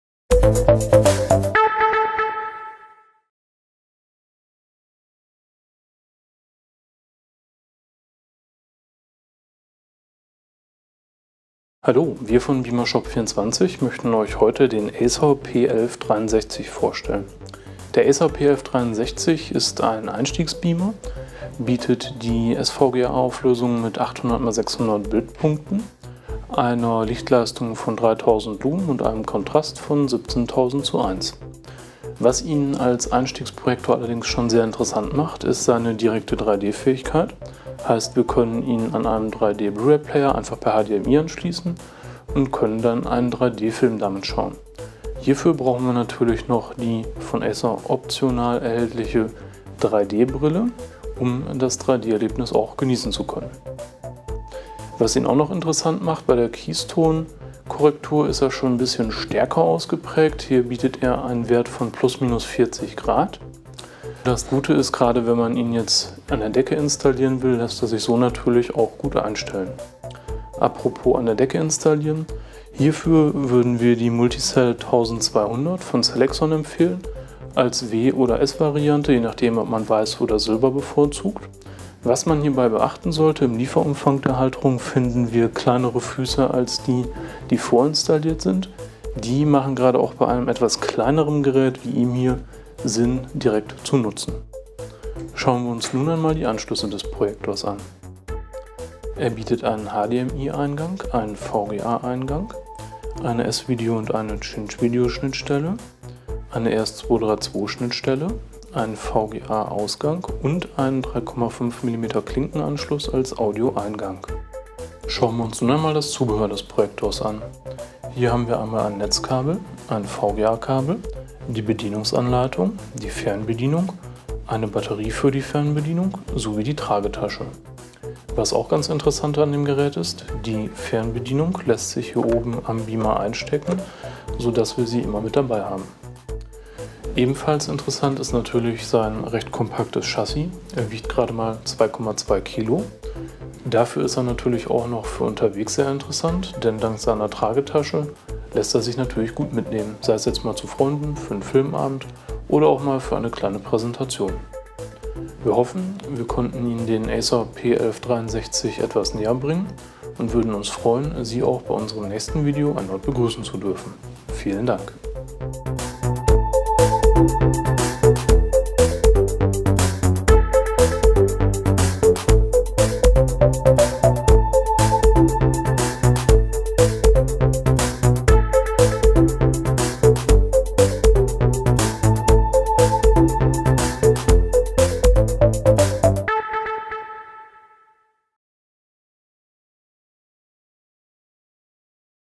Acer P1163 - 3D zum Einstiegspreis
Der Acer P1163 bietet als Einstiegsprojektor bereits echtes 3D! Dank seiner sehr ansprechenden Preis / Leistung bietet er sich gerade für den Einstieg in die Beamerwelt an, sei es für Präsentationen oder auch zu Hause für den 3D Film Abend!

Der Acer P1163 bei Beamershop24!